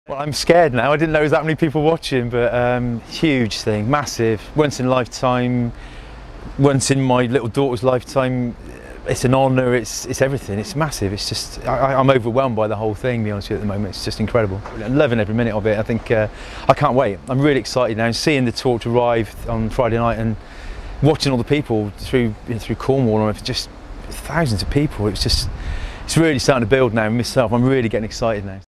Robert Berry Olympic Torch Bearer
Robert Berry from Hanham, Bristol on being chosen as an Olympic torch bearer